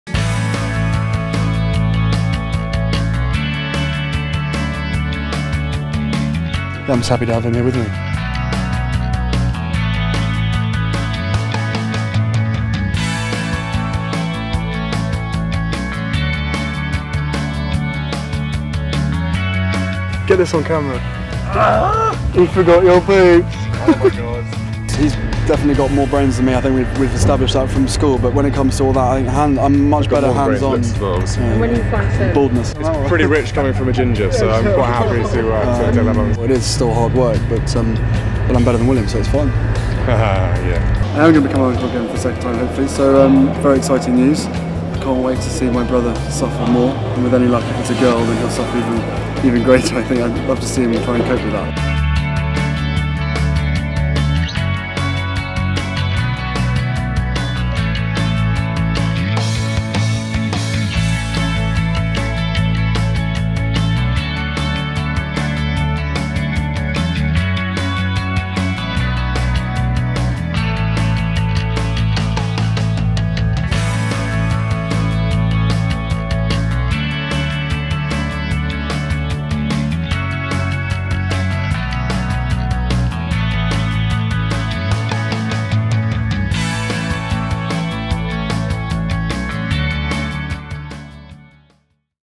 Prince William and Prince Harry’s Cutest Brother Moments | Harper's BAZAAR
Take a look back at some of Prince William and Prince Harry's adorable brotherly moments.